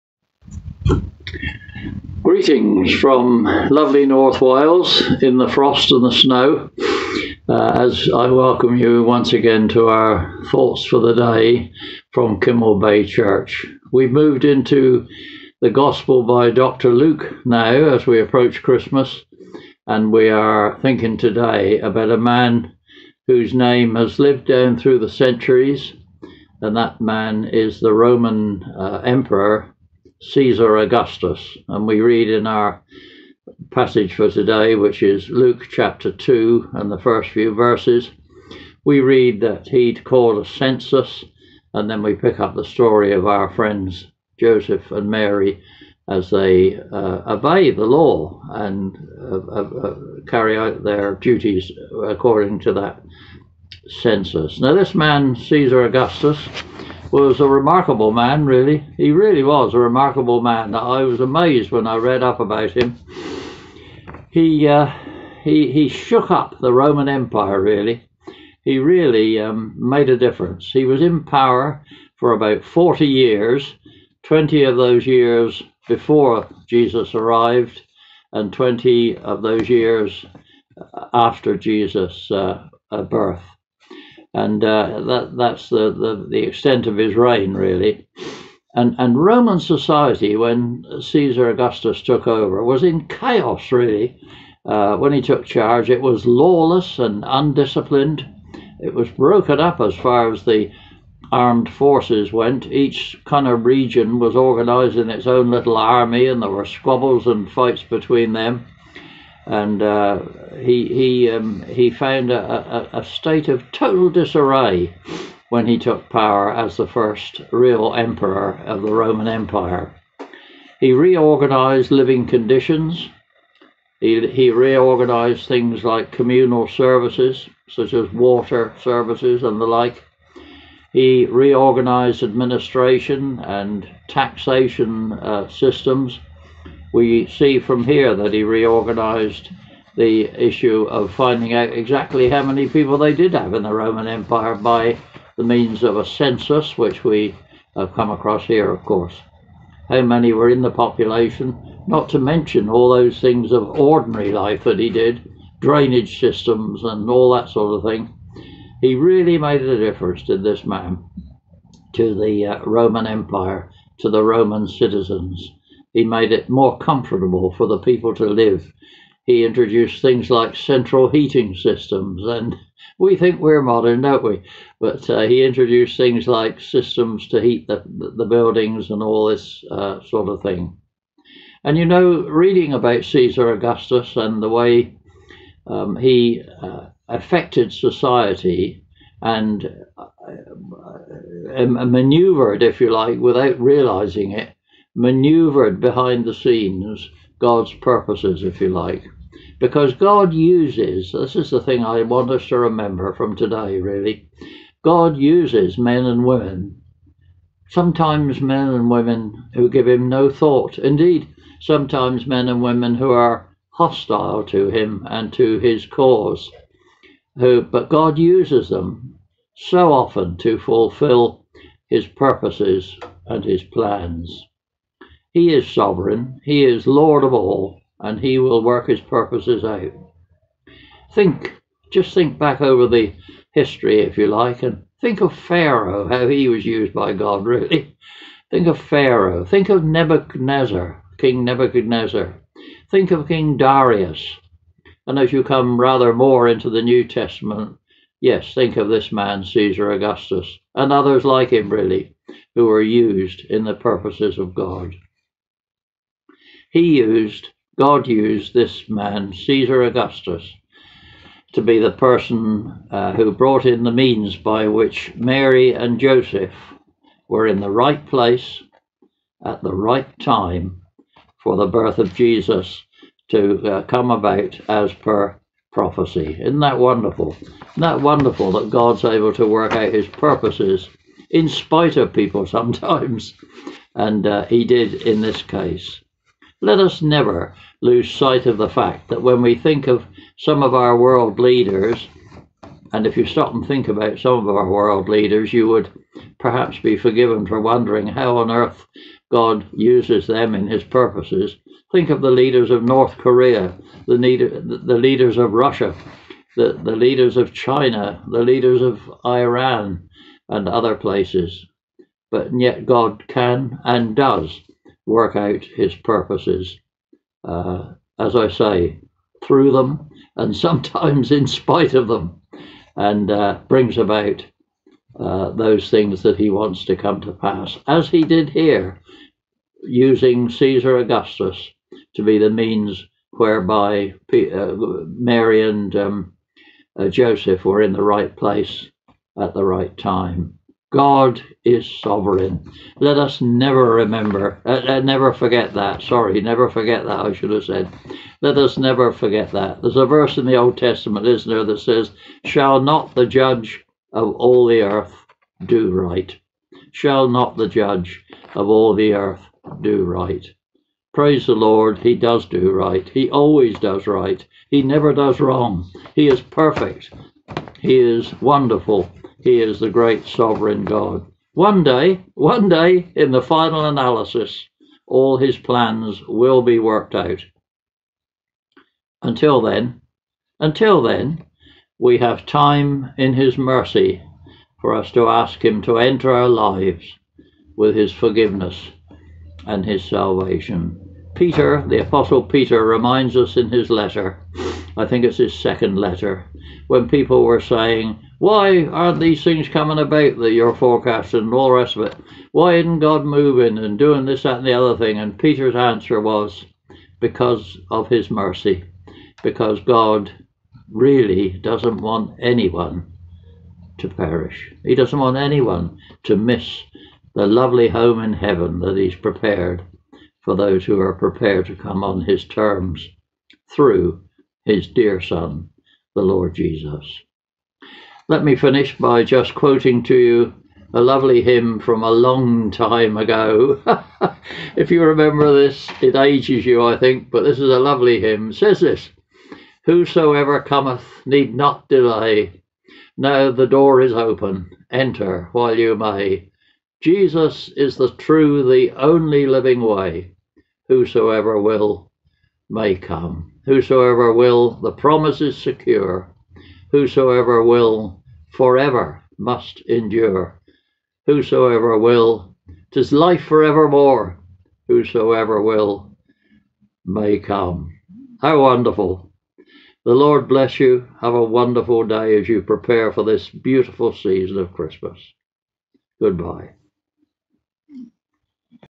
Characters around the Cradle
In today’s vlog Trev is looking at the Roman leader called Ceaser.